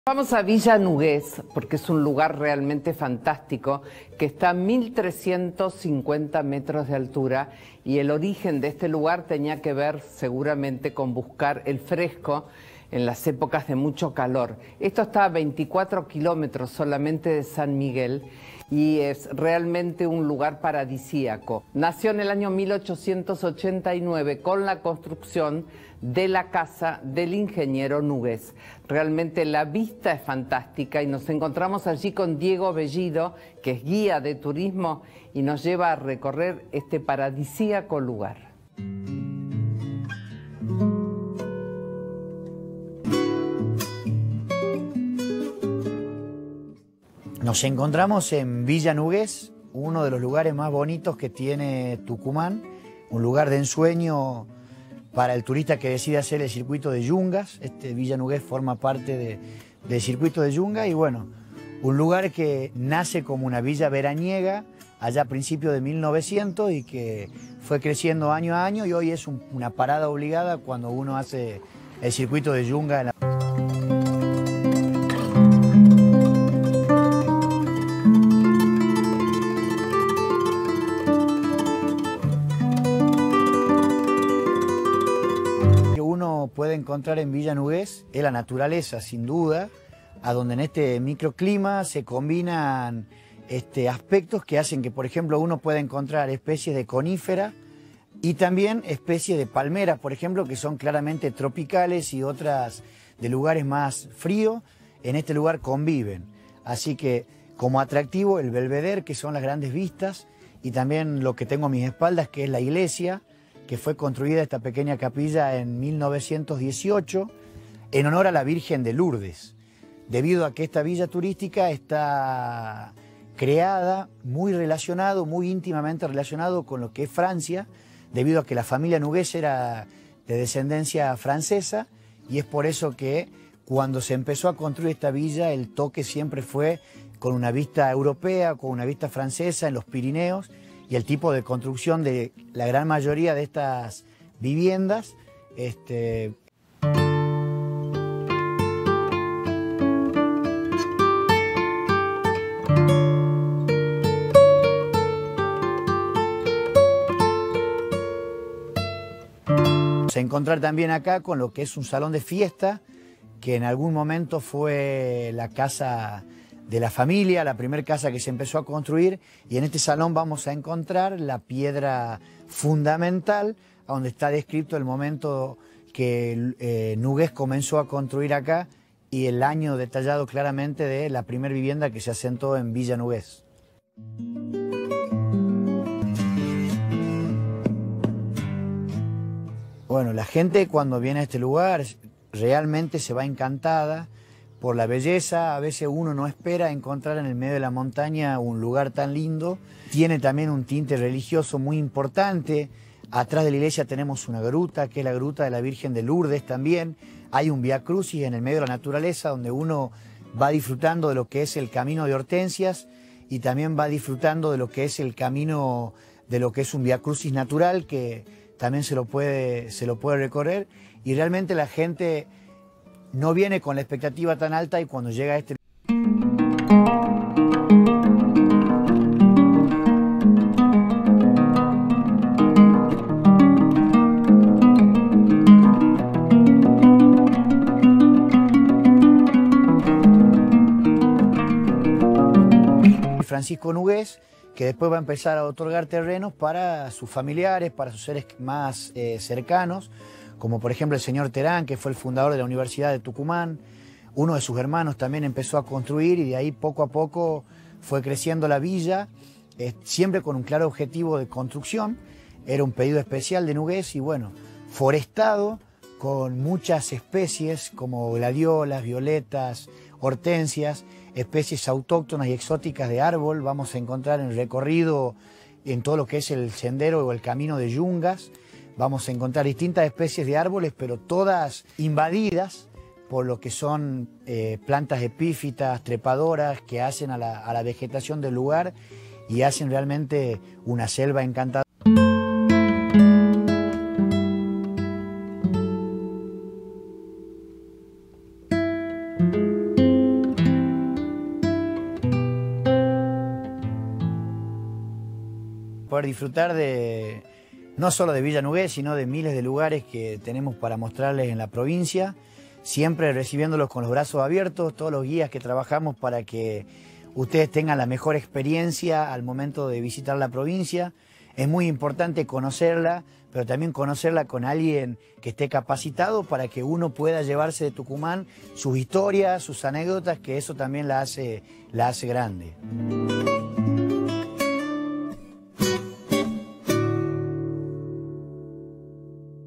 Villa Nougues Tucumán su HISTORIA y CURIOSIDADES
en este video recorreremos los paisajes de Villa Nougues, un paraíso en medio de la selva tucumana